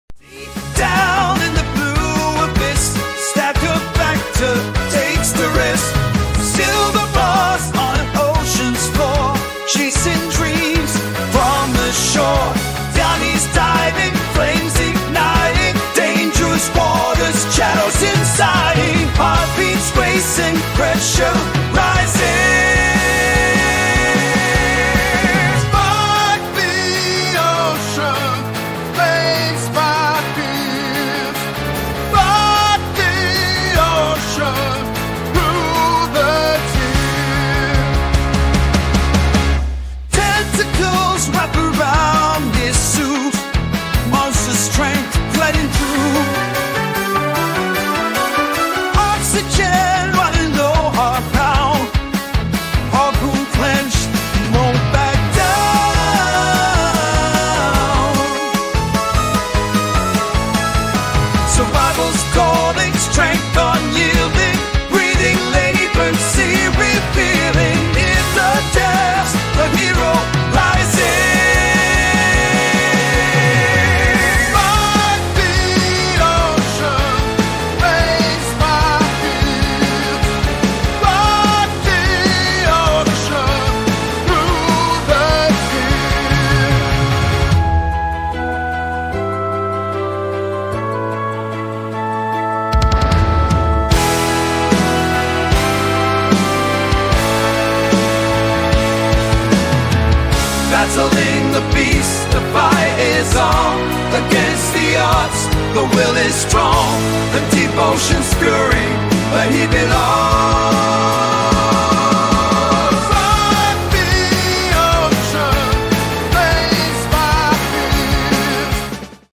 🔊🔊 2019 Diver & Octopus / Postal Express Mint | Music By: Bankster Nation 🔊🔊
Year: 2019
Title: Diver & Octopus
Mint: Postal Express Mint
Mintage: PR 35, EN 35, SEN: 12
1oz .999 Silver Bar